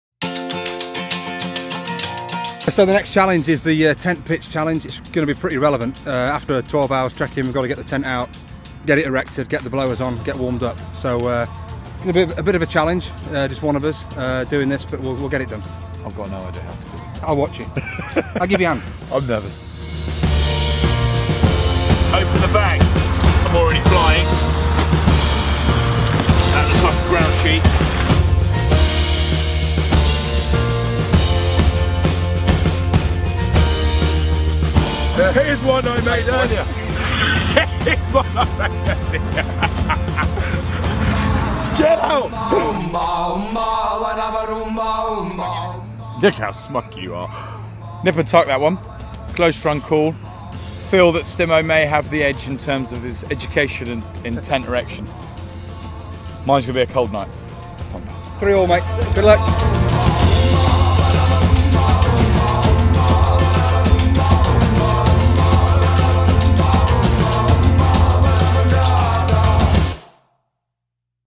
ArcticChallenge TeamTim V TeamOllie Challenge 6
With our ArcticChallenge fast approaching in 15 days time, this next challenge see's our Team Captains putting their Arctic training to the test...by putting up tents. Marvel at the skill involved.